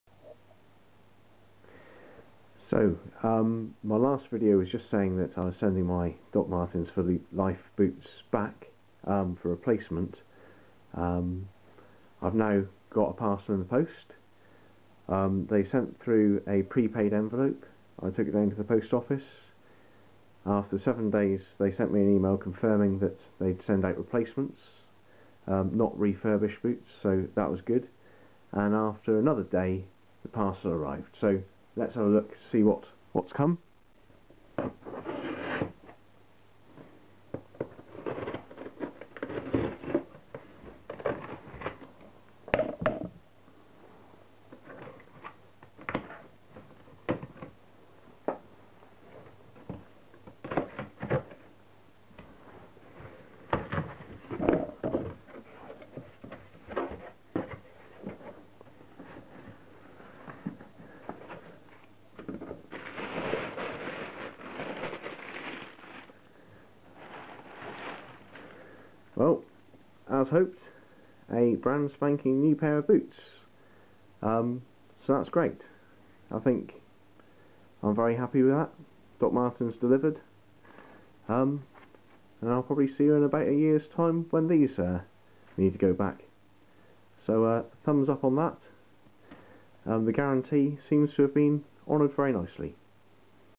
Dr Martens For Life 1460 "Tan" Boot 1 YEAR REVIEW *GUARANTEE UPDATE*
I am pleased to say that Dr Martens delivered on their guarantee promise. Providing a prepaid postage label, and new boots in 8 days from posting.